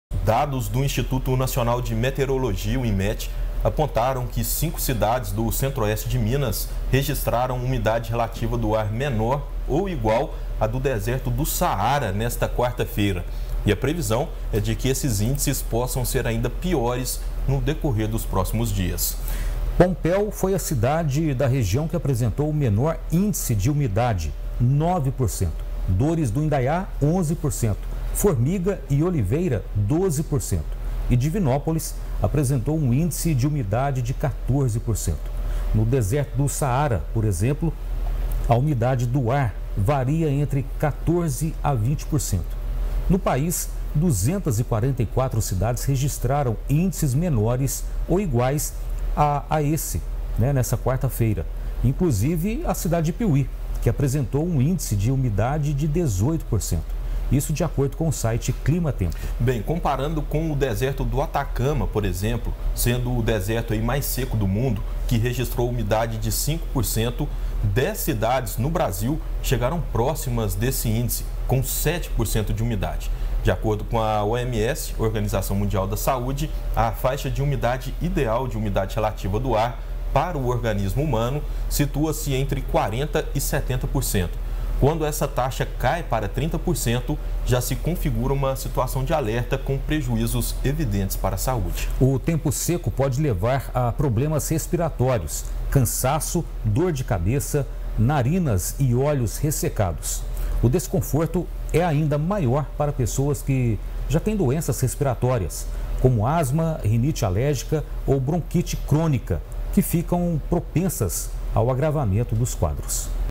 CIDADES DO CENTRO-OESTE DE MINAS REGISTRAM UMIDADE IGUAL AO DESERTO DO SAARA
| REGIÃO |

Dados do Instituto Nacional de Meteorologia (INMET) apontaram que cinco cidades do centro-oeste de Minas Gerais registraram umidade relativa do ar menor ou igual à do Deserto do Saara nesta quarta-feira, e a previsão é de que esses índices possam ser ainda piores no decorrer dos próximos dias. Saiba mais detalhes: